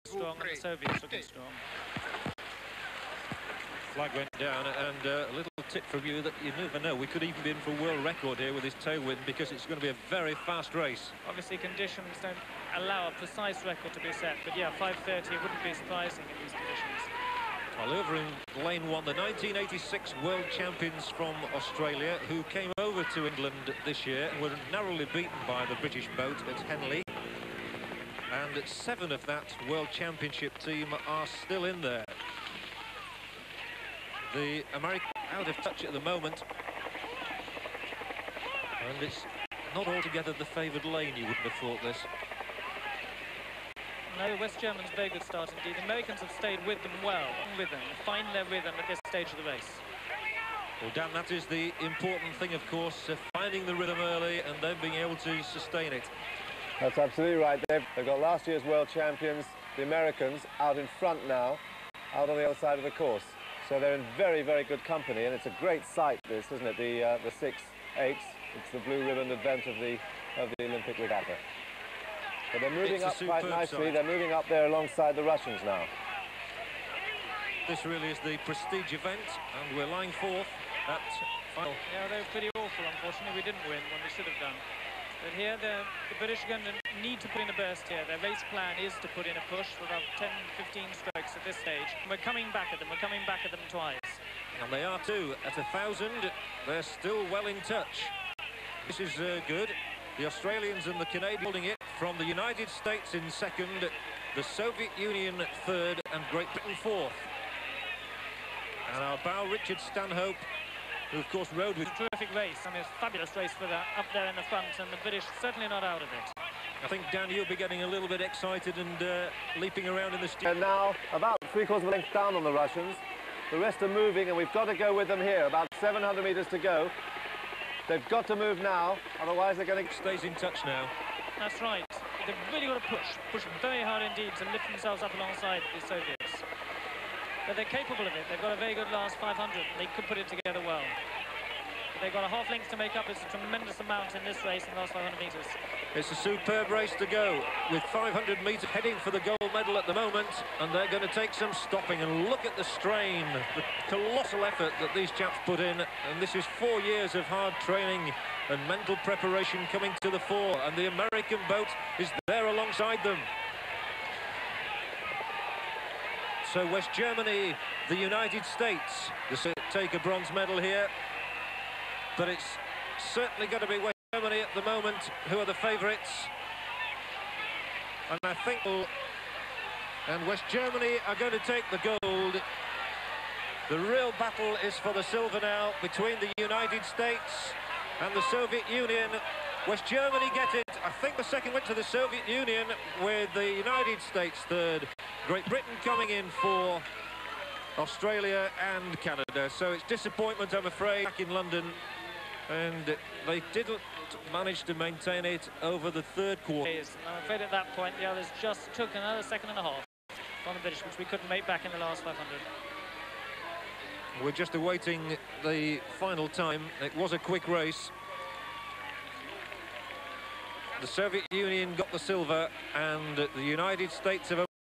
1988 Seoul Olympics rowing Mens 8 Final
Seoul Olympics rowing mens eight final
Lane 1 Australia
Lane 2 USA
Lane 3 West Germany
Lane 4 Soviet Union
Lane 5 Great Britain
Lane 6 Canada